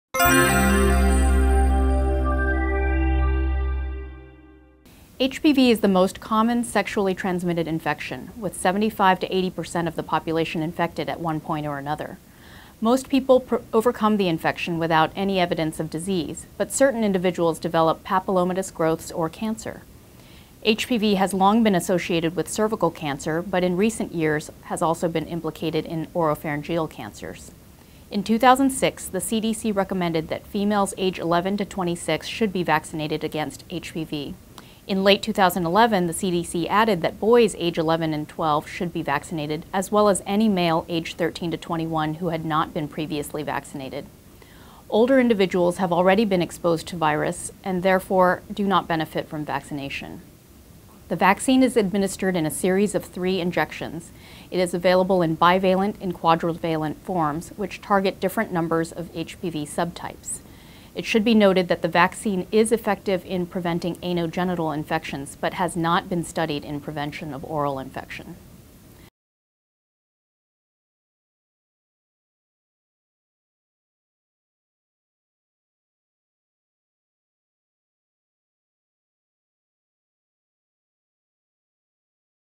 HPV Vaccination CME 2Go Dr. Marita Teng of Mount Sinai
Dr. Teng, ENT/Head and Neck Cancer Surgeon at Mount Sinai in New York explains who is a candidate for HPV vaccination.